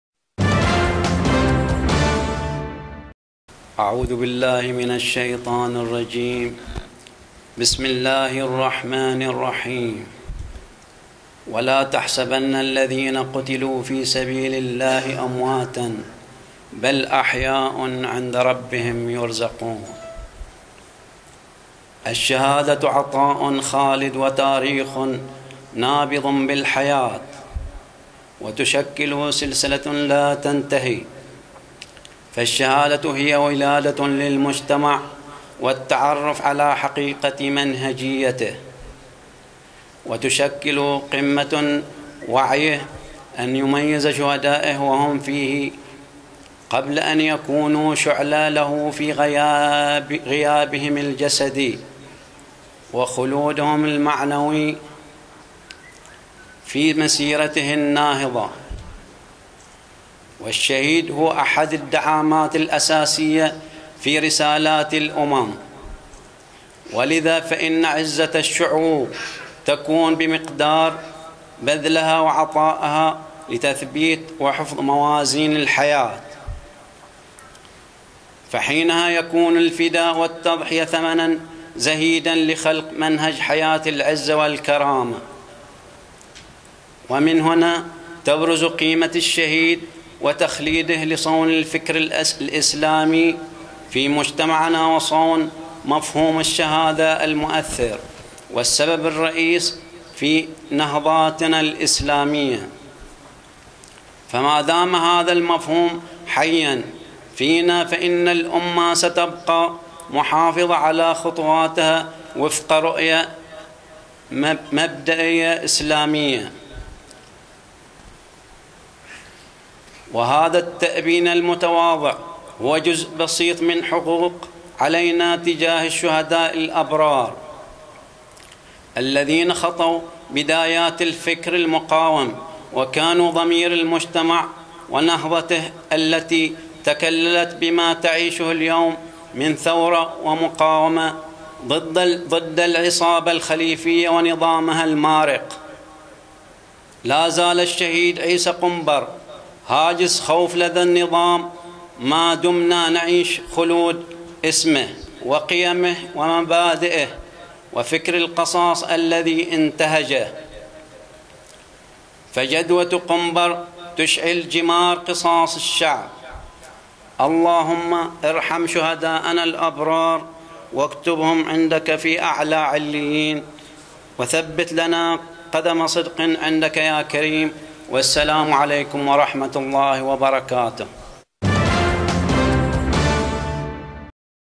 كلمة عوائل شهداء النويدرات - تأبين شهداء قرية النويدرات Bahrain
كلمة عوائل شهداء النويدرات في تأبين شهداء النويدرات الذي اقيم مساء أمس الثلاثاء (25 مارس/آذار 2014) بمأتم آل زيد في قرية النويدرات وفاءاً للشهداء وتخليداً لذكراهم.